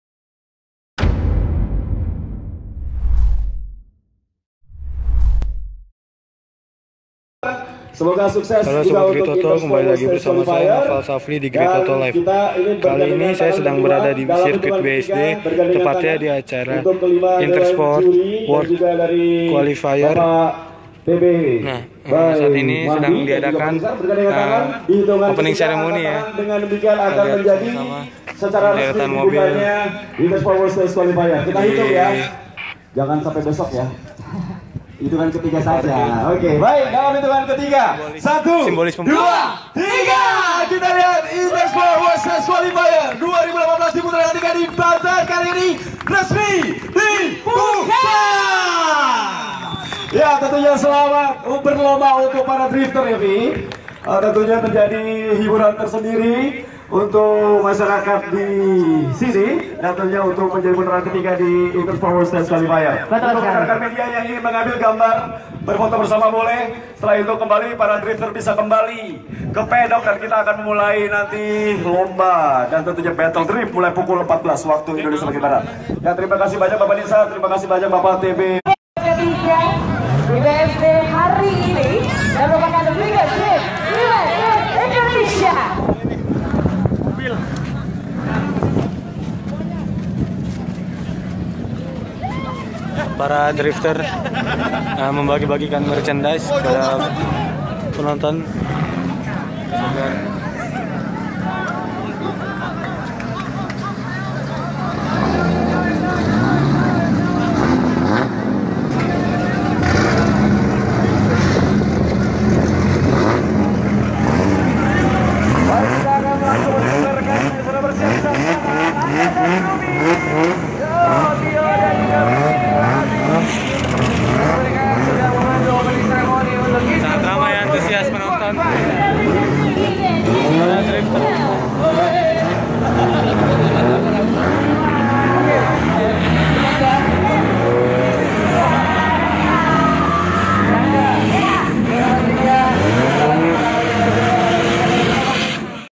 Opening Ceremony Intersport World Stage Qualifier BSD
Halo pemirsa GridOto.com

Saat ini GridOto.com sedang berada di sirkuit BSD.

Di tempat ini sedang berlangsung opening ceremony Intersport World Stage Qualifier.

Opening dibuka oleh perwakilan IMI dan perwakilan Intersport.

Mau tau bagaimana situasinya? Simak video berikut ini

Reported by Naufal Shafly Dimas

*Video diambil dari Instagram Live GridOto